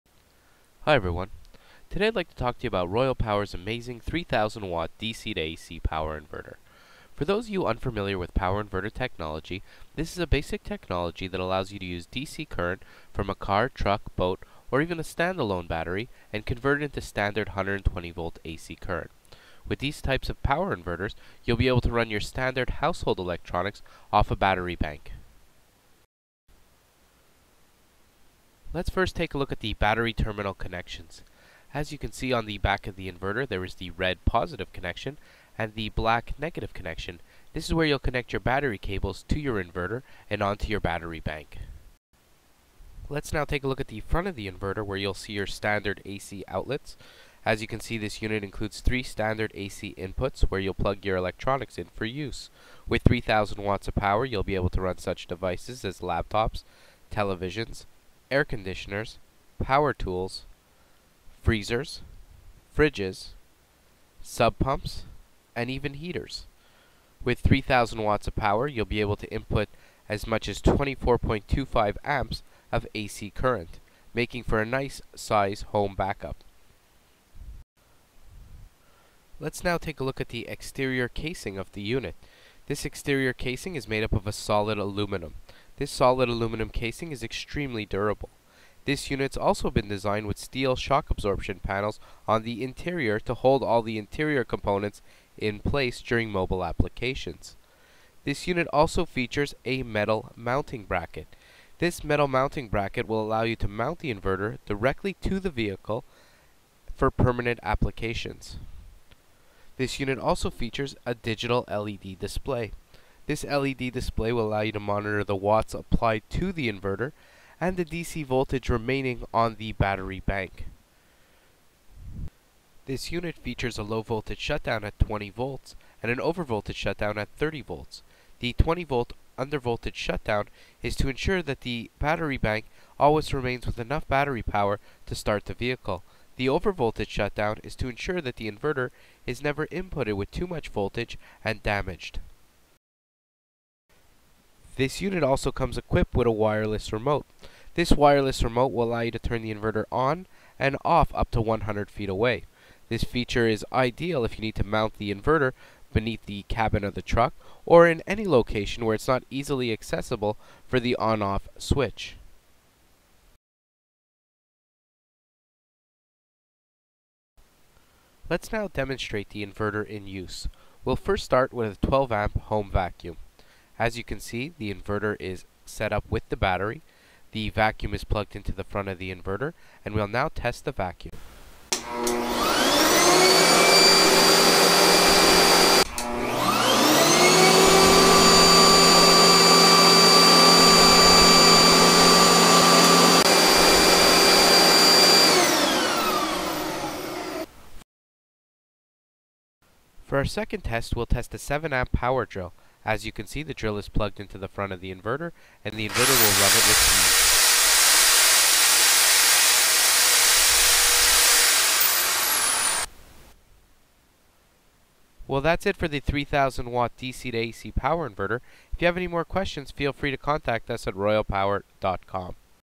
3000 Watt 24 Volt DC to 110 Volt AC Power Inverter
The (PI-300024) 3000 watt 24 volt dc to 120 volt ac power inverter takes a 20 to 30 volt direct current power source and inverts the current into 120 volt alternating current. This power inverter will allow you to operate up to 24.25 amps of electrical devices. Outputting modified sine wave power allows you to run all types of standard household electronics. This DC to AC power Inverter includes a wireless remote system which allows you to turn the inverter on and off up to 100 feet away. The digital LED monitor allows you to view the battery voltage and watts applied to the unit. This size power inverter can handle a large range of heating and cooling systems and power tools.

-3000 watts continuous power
-6000 watts peak power
-Provides 24.25 Amps
-DC input voltage: 20-30V
-Anodized aluminum case provides durability
-Built-in Cooling Fan
-Internal, Replaceable 30 Amp spade-type Fuses
-3 3 Prong 120 volt AC outlet
-Digital Amp~Wattage display Helps monitor overloads
-Overload Indicator
-Power ON/OFF Switch
-Free 100ft Range Wireless Remote
-3 FT Battery Cables Included